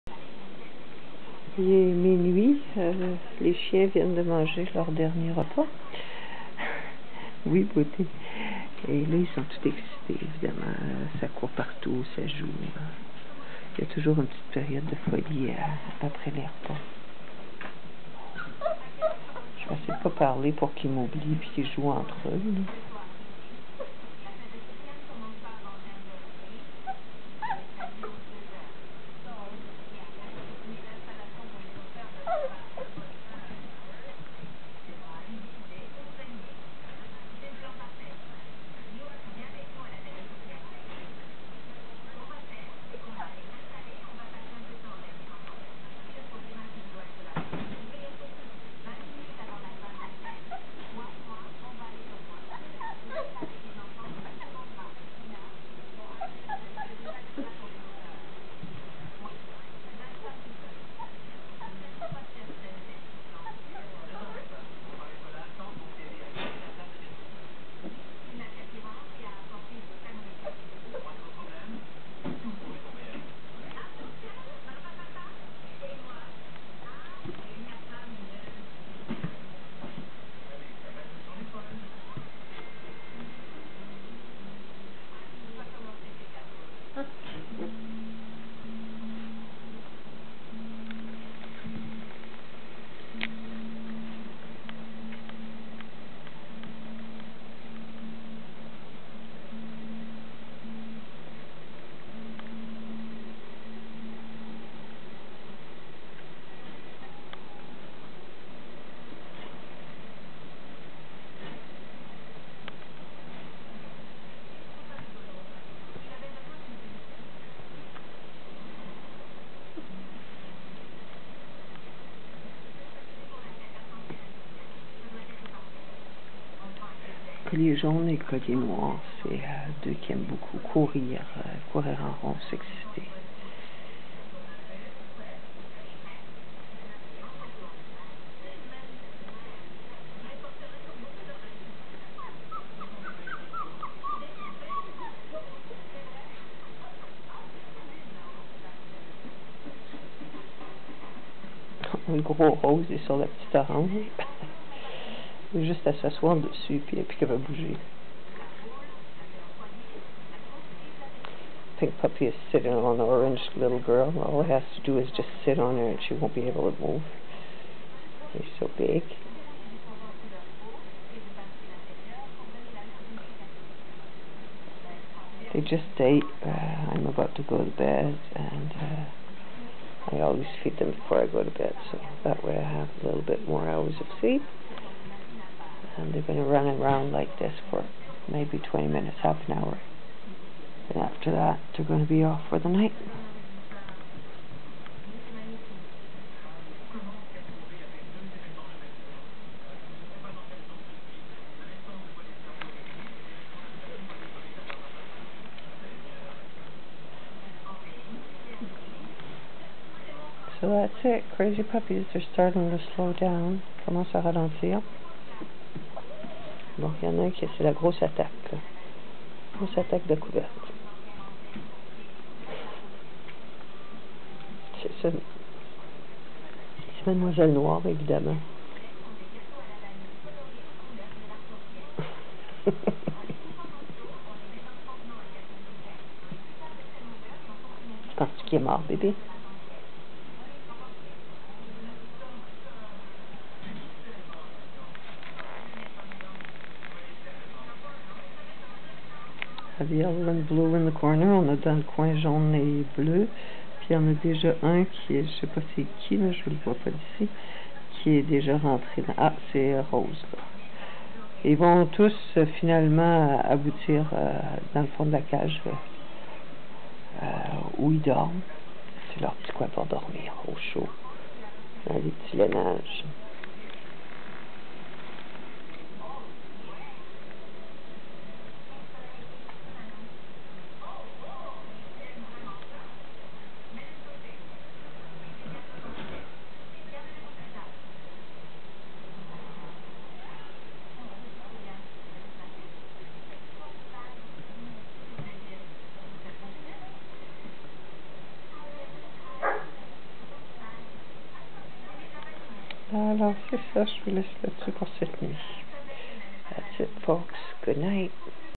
Berger Picard Canada - Noble Picard 2013 puppies - Deva X Gaspar
Petits jeux dans le parc après le dernier repas tard hier soir.
Play time in the pen after their midnight snack.